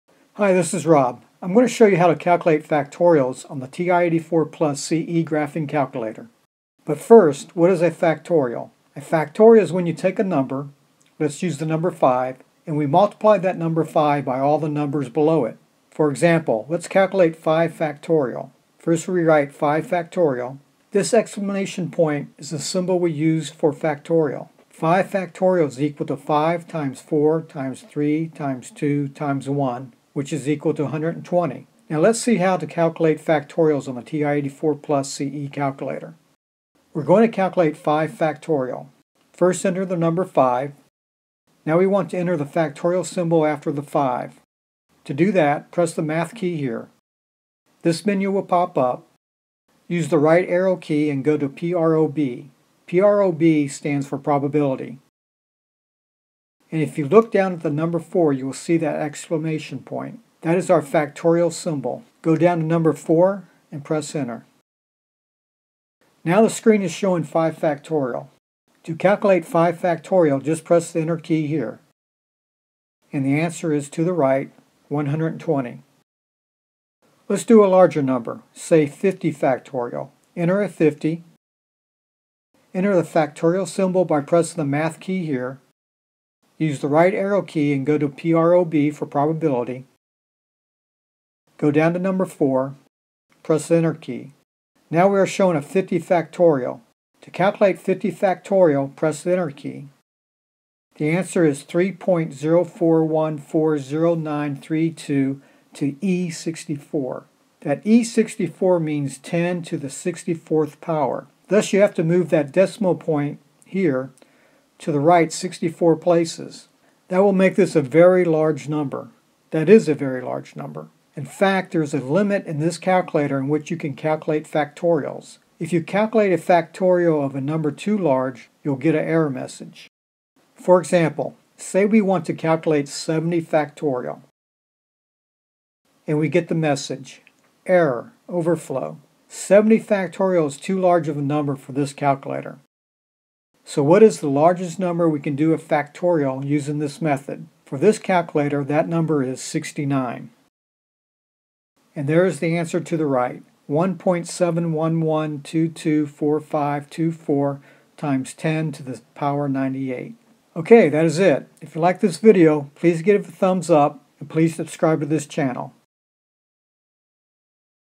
How to Calculate Factorials on the Ti 84 Plus CE Calculator
Calculate Factorials on the Ti 84 Plus CE Color Graphing Calculator.
Factorial problems on a calculator.